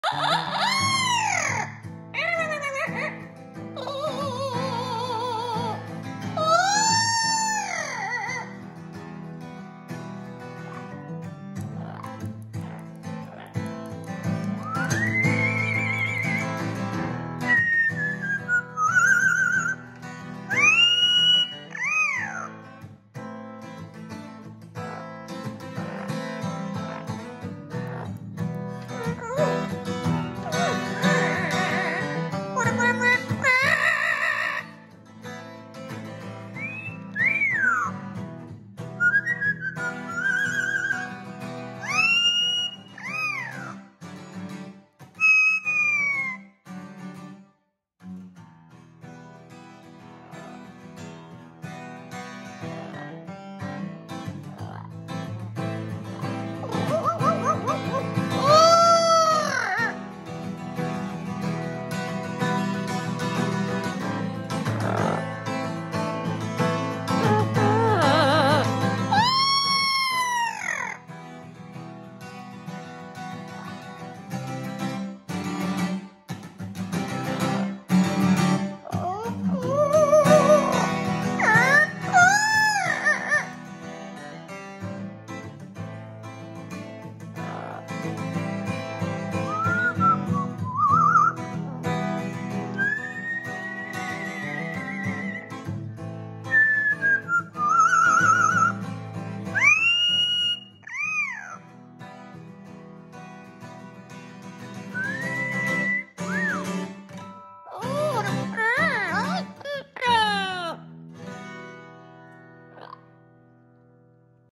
1981 I wrote this
1981 this chord progression got stuck in my head and I’ve never really took it beyond what you hear. I know Tico likes making up the lyrics to it.